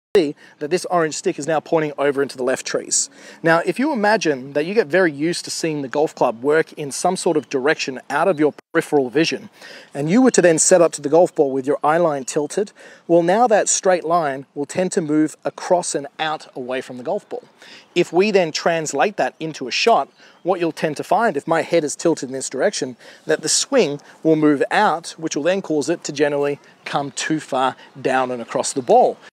The Effects of Your Eye Line | Address Position
How are you position your head at address can have a huge influence on the direction of your eyeline and therefore the movement of the club head relative to what feels comfortable.